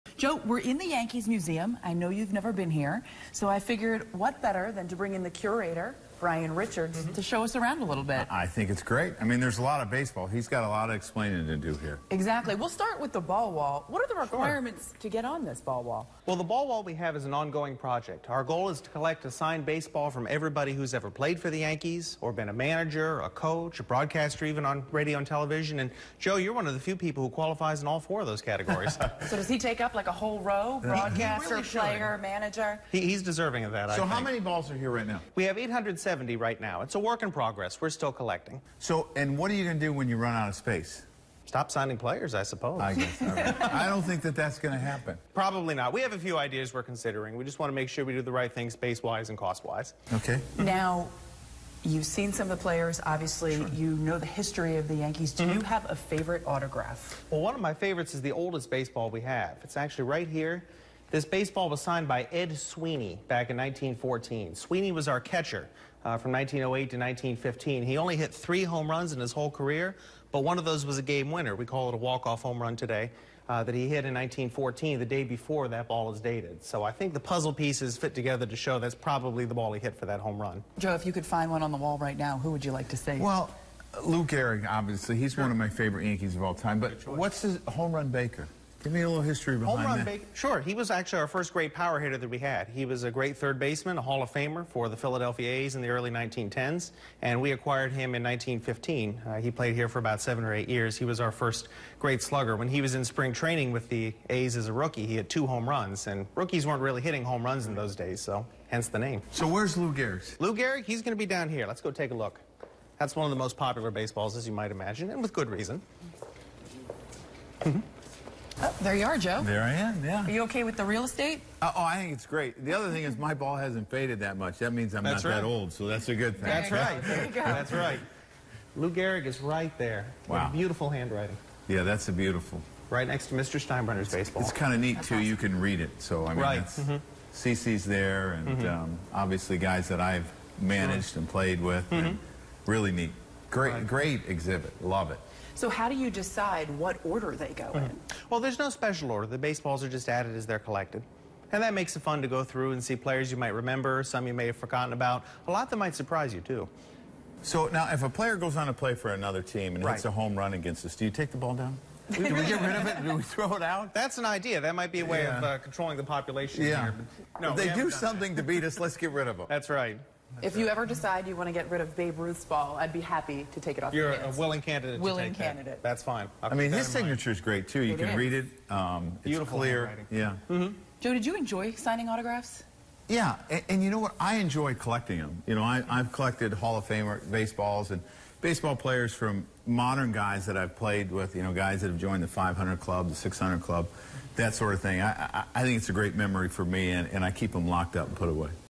Joe Girardi visits the Yankees Museum
Joe Girardi and Meredith Marakovits check out historic Yankees memorabilia, including the wall of autographed balls, in the Yankees Museum at Yankee Stadium.